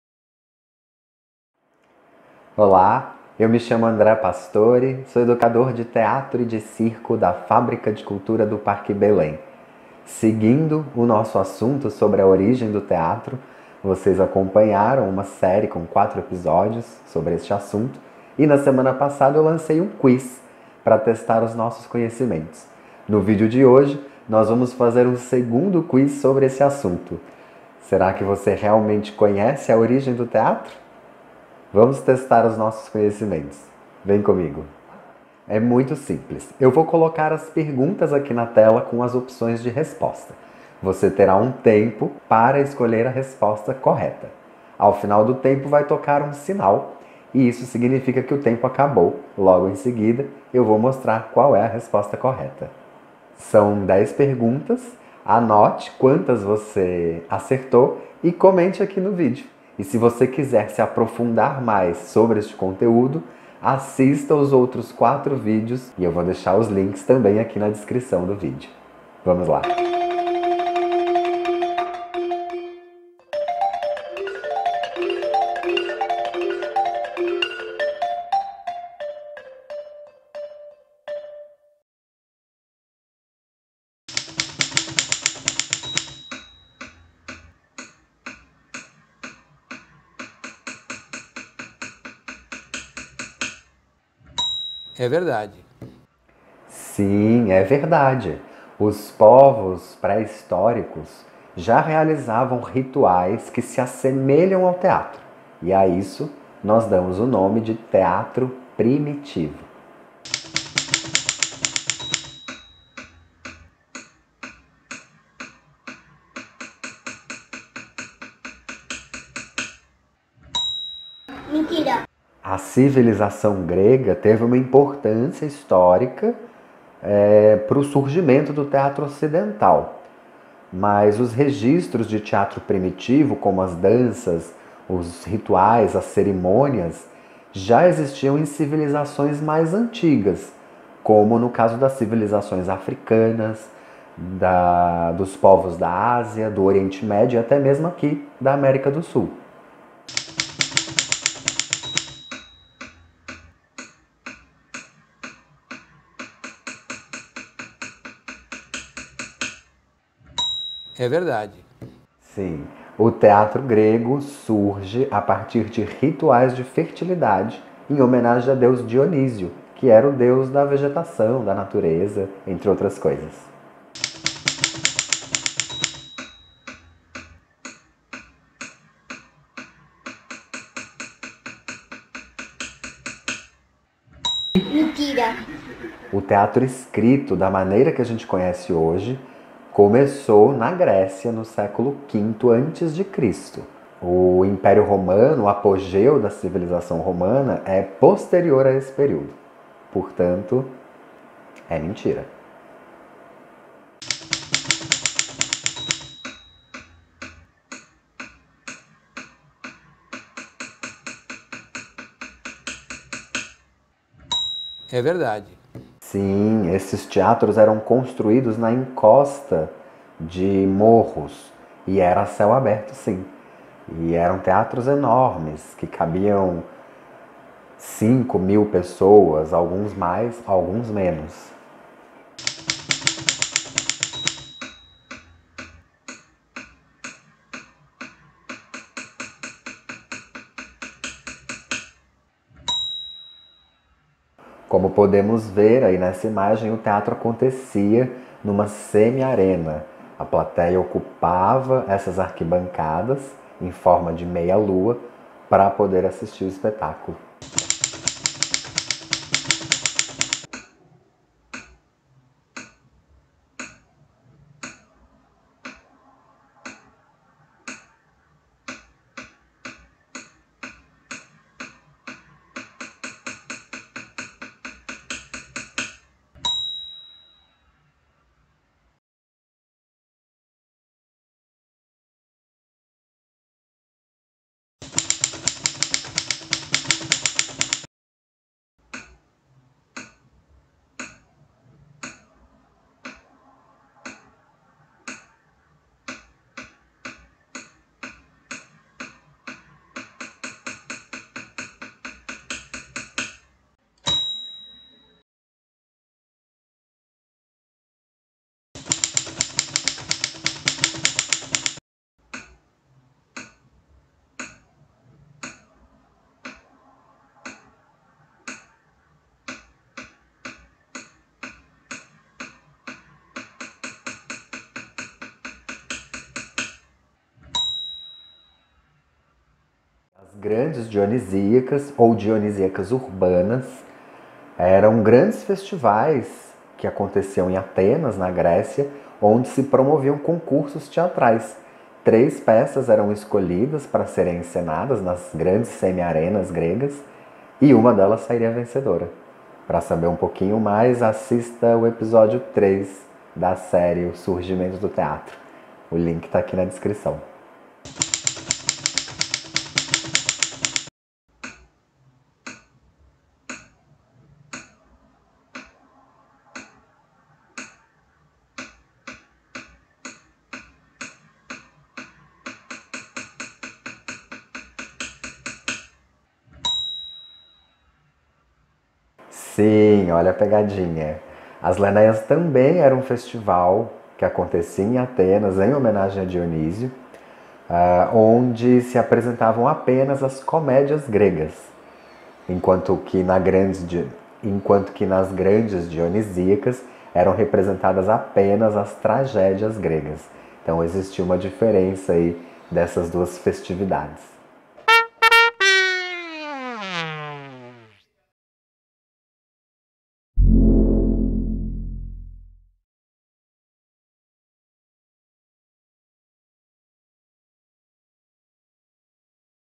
Aula: QUIZ II do Teatro (FC Belém)
Vamos testar nossos conhecimentos sobre a origem do teatro em mais um Quiz? Desta vez, com mais desafios e uma pitada de humor.
Teatro Primitivo: dos povos nômades às civilizações
A civilização grega e o Ditirambo
As Grandes Dionisíacas e a Arquitetura do espaço cênico
Função da Tragédia e Comédia grega